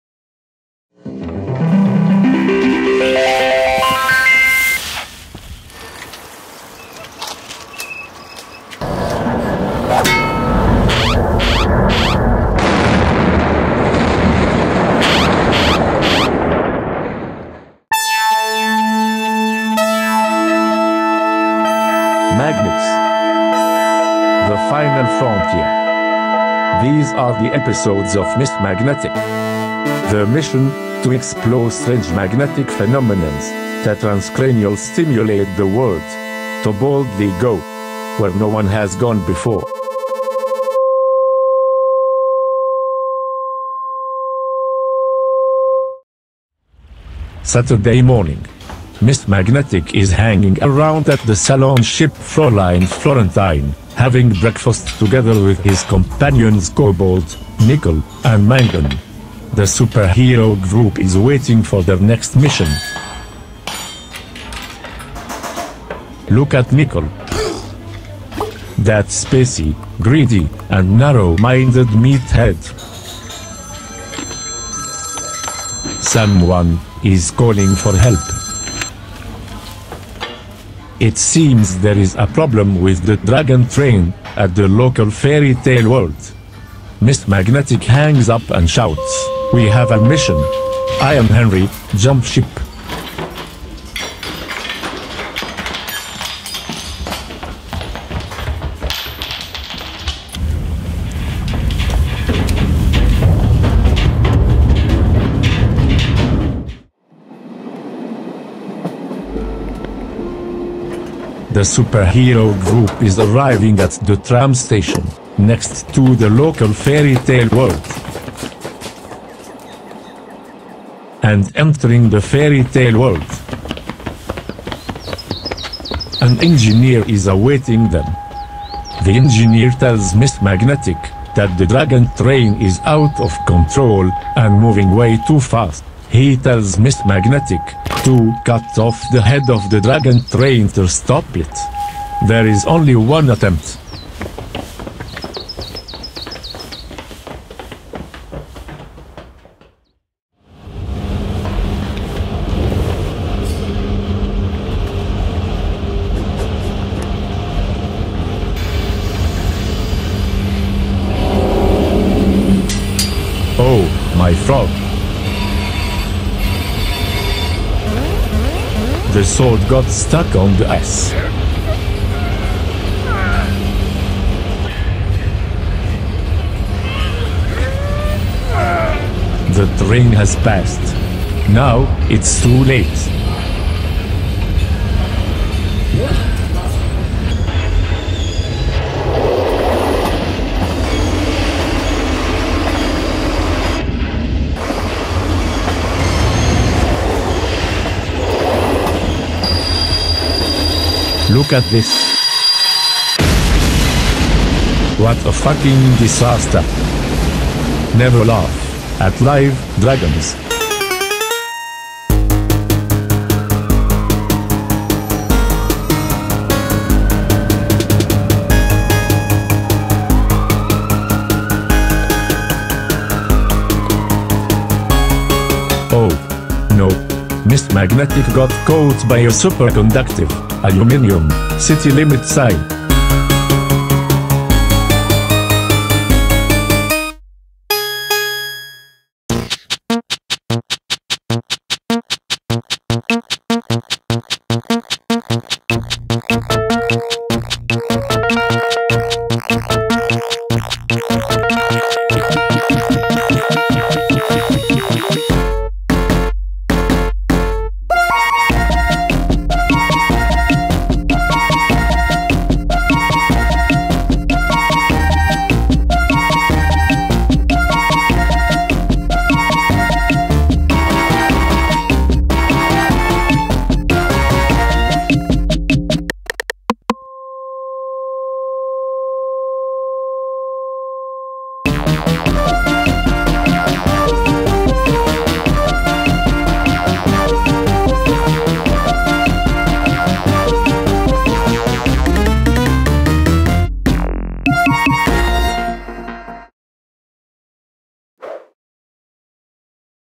MISS MAGNETIQ - EPISODE I: THE STEELCITY AS ELECTROMAGNETIC FIELD (PART III)
If the wagon is a dragon! MISS MAGNETIQ did it again and ruined another part of a town in Austria. This is the third video out of series taking place at the local fairytale cave in Linz.